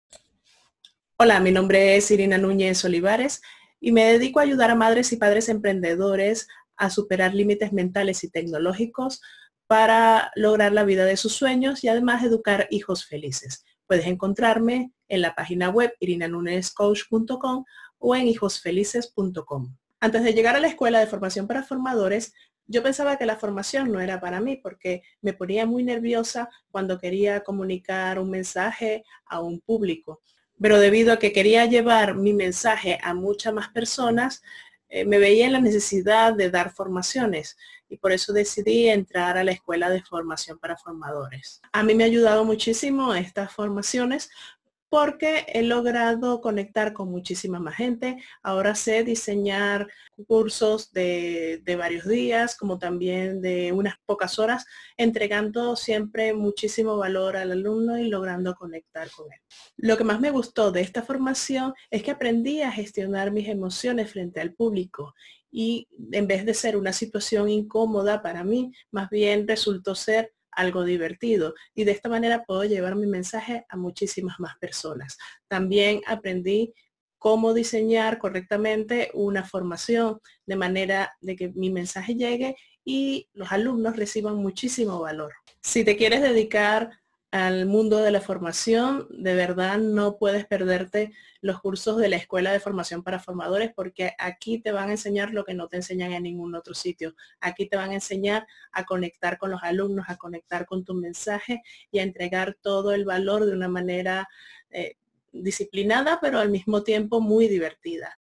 Irina Nuñez
Antes de conocer la escuela, yo pensaba que la formación no era para mí porque me ponía muy nerviosa cuando quería comunicarme con un público. Pero como quería llevar mi mensaje a mucha más gente me vi en la necesidad de dar formaciones y por eso me apunté.
A mí me ha ayudado muchísimo. Ahora sé diseñar cursos de varios días o de pocas horas entregando siempre muchísimo valor al alumno y conectando con él.
Lo que más me gustó es que aprendí a gestionar mis emociones frente al público, ahora en vez de ser una situación incómoda es algo divertido.
Si te quieres dedicar al mundo de la formación, de verdad no puedes perderte los cursos de la escuela de formación para formadores porque aquí te van a enseñar lo que no te enseñan en ningún otro sitio.
Te van a enseñar a conectar con tus alumnos, a conectar con tu mensaje y a entregar todo el valor. Y de una manera disciplinada pero también muy divertida- dice Irina Núñez, alumna de nuestro máster.